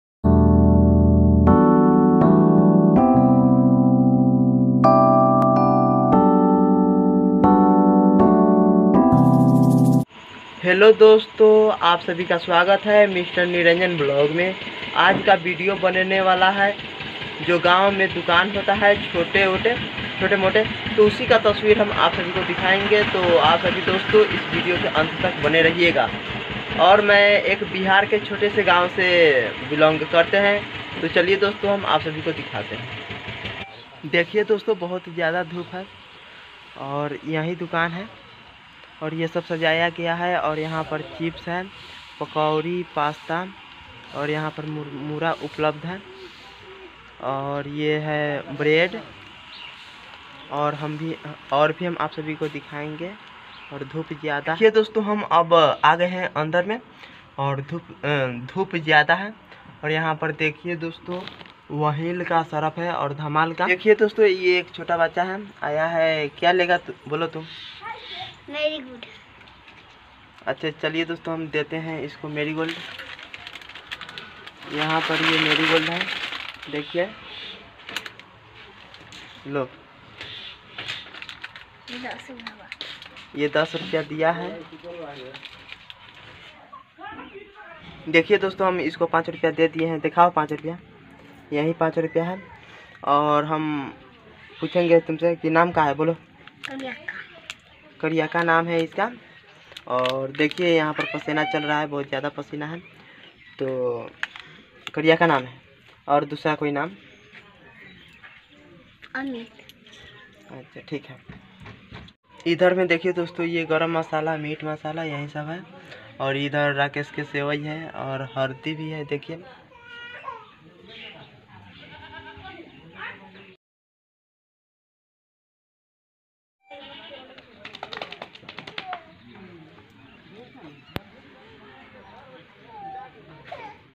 Village In Dukan Bihar Vlog #2022
bihar ke ak chhote gaav me Dukan huaa hai.